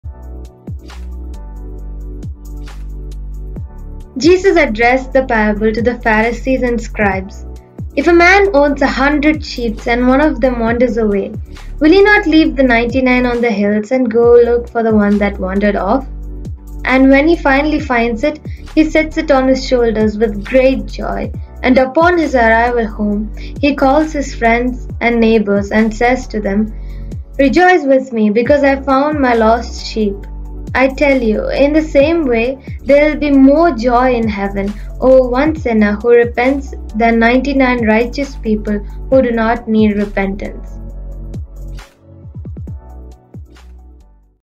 The Parable of the Lost Sheep | Animated series
This is a short animated video of one of the parables of Jesus, The Lost Sheep.

⭐Repent now and be cleansed so that you become the sheep that Jesus celebrates for⭐

🔎Find the portion: Matthew 18:12-14